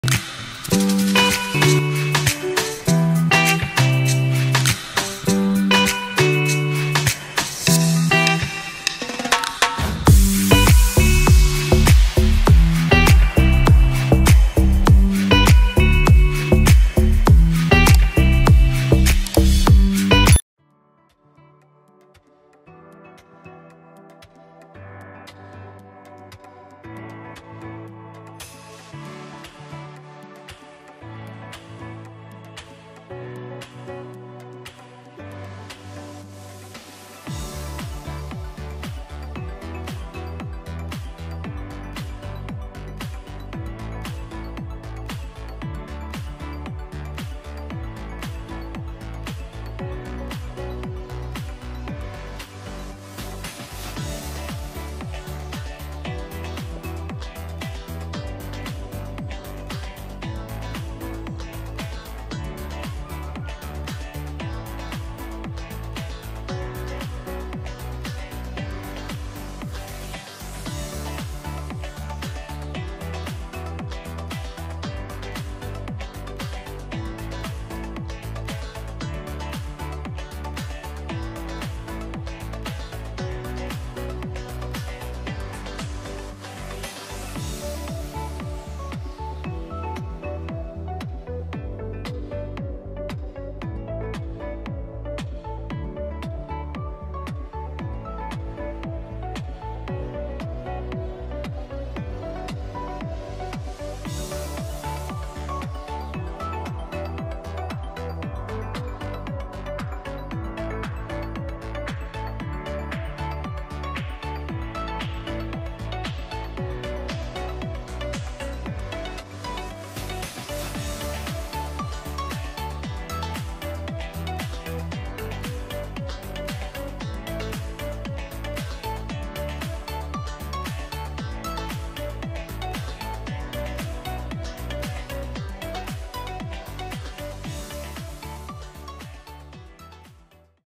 Welcome To Bloxburg Is Now Free?! Here Are My Thoughts
I cannot believe Bloxburg is free! When I first heard this I thought it was a joke! Here are my opinions, comment yours!